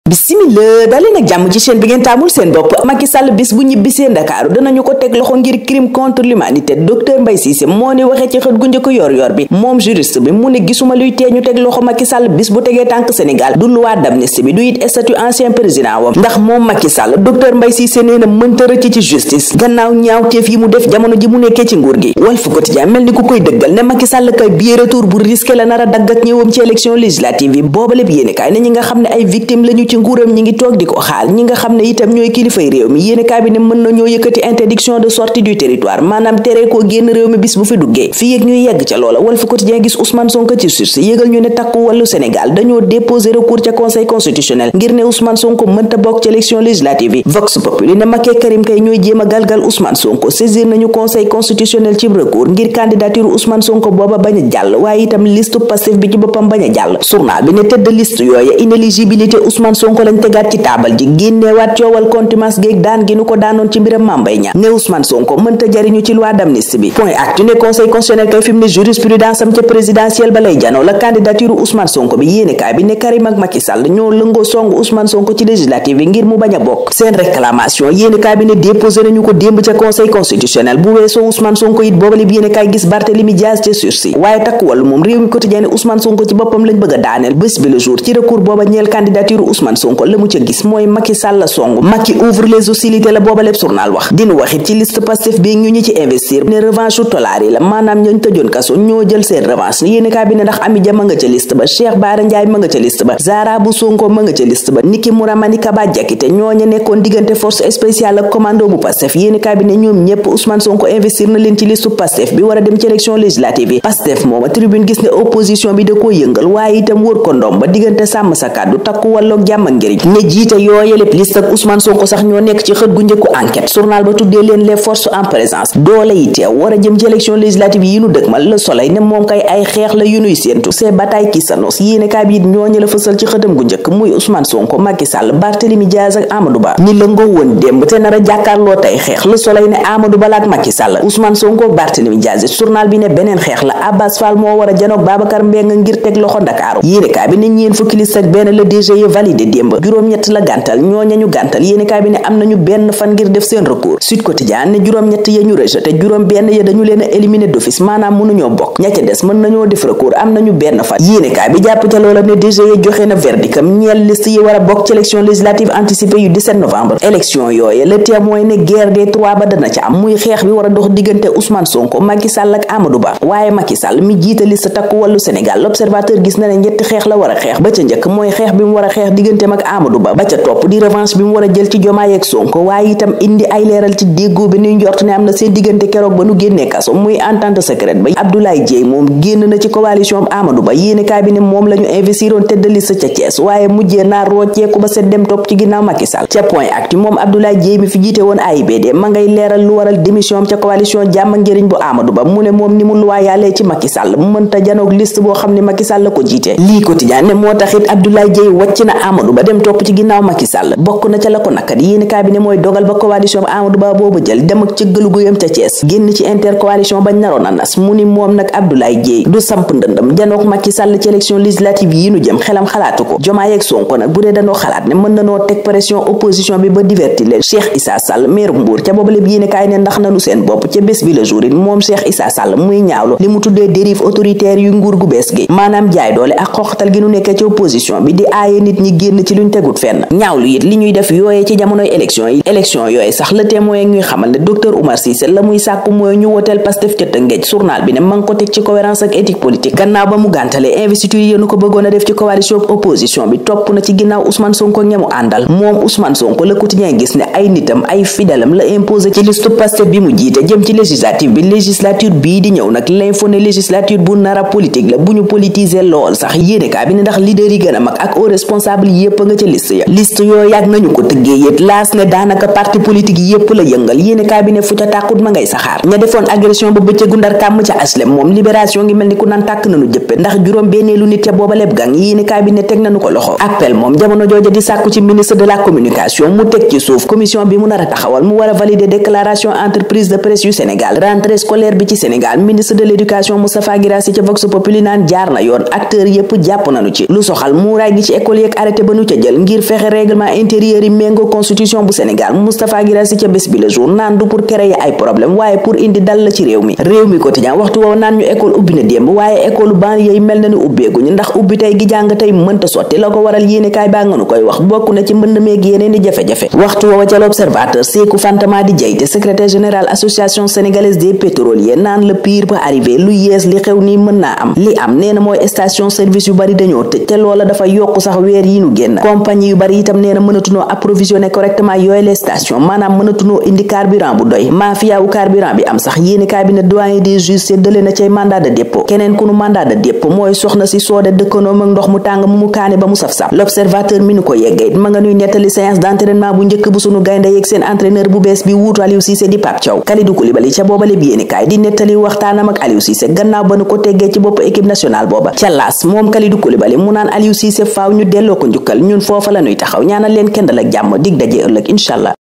Macky risque d'être arrêté par...SONKO mise sur Mère Amy Dia,Cheikh Barra, Maimouna Bousso et Cie..
Macky risque d'être arrêté par...SONKO mise sur Mère Amy Dia,Cheikh Barra, Maimouna Bousso et Cie..Revue de presse du 8 octobre 2024